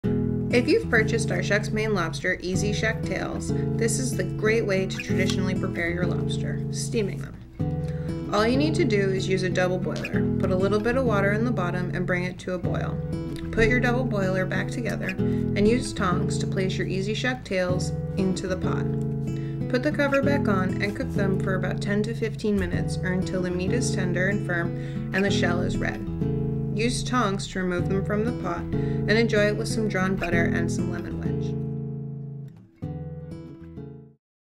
How to Prepare Steamed Maine Lobster Tails
Step by step instructions on how to prepare your Shucks Maine Lobster EZ Shuck Tails.